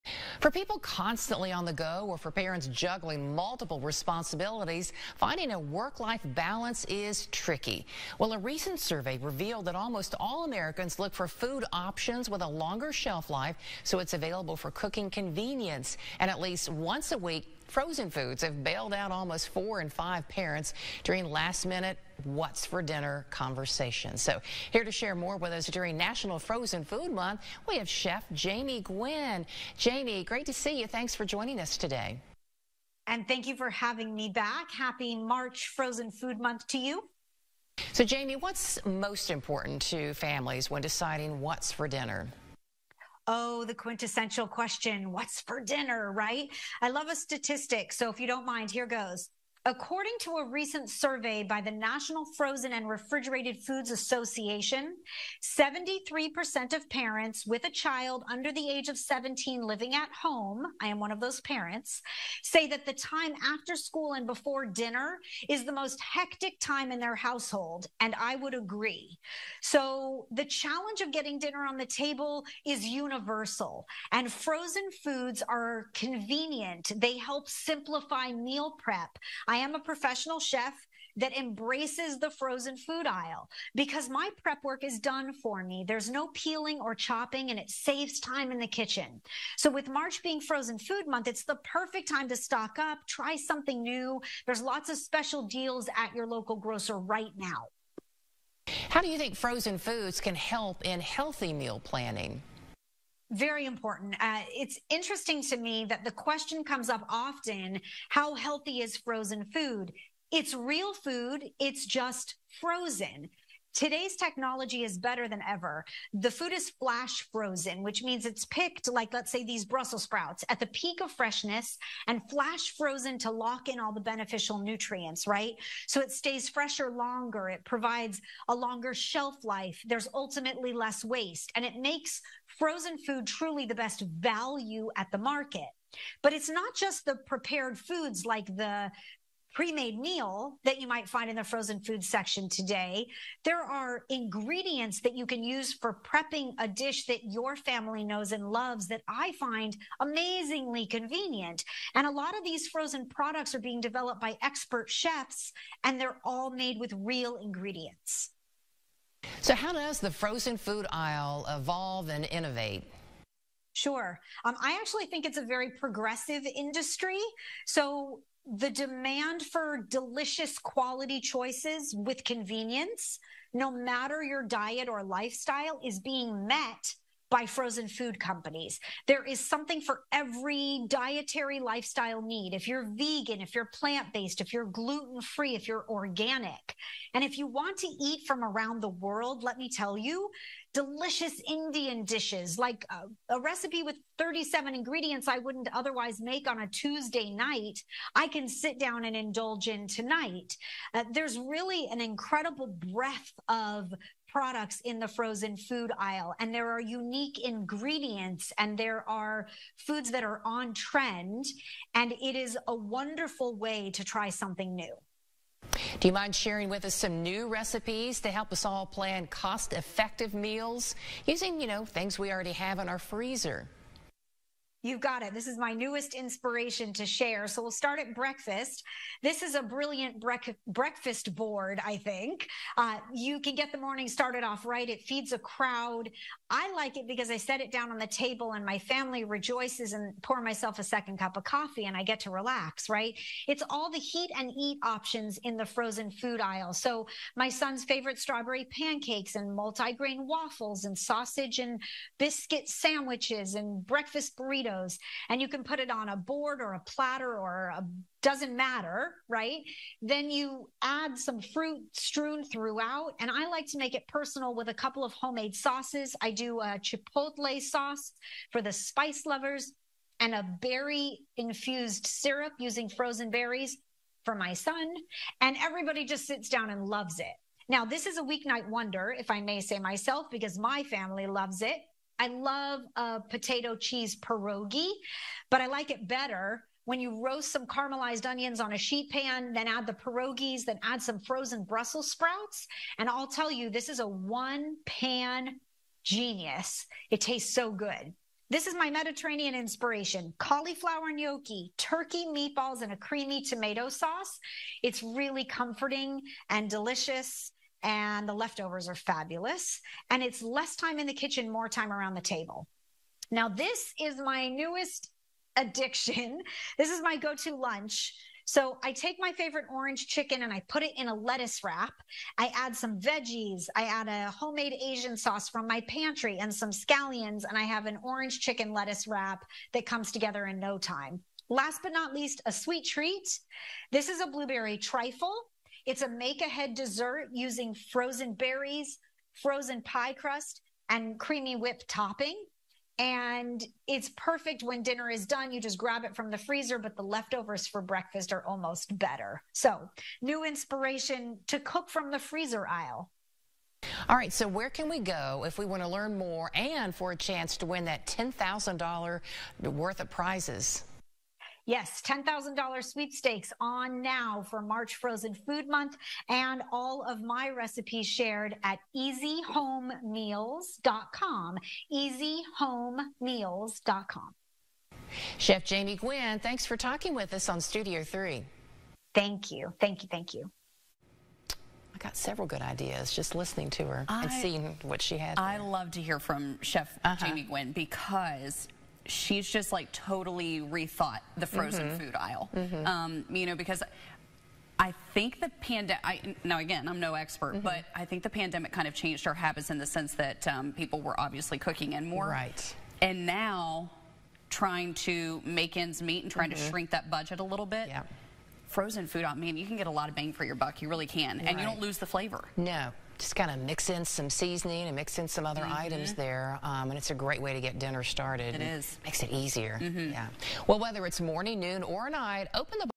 National Frozen Food Month
Chef Jamie Gwen shares frozen food items that will not sacrifice flavor or nutrition.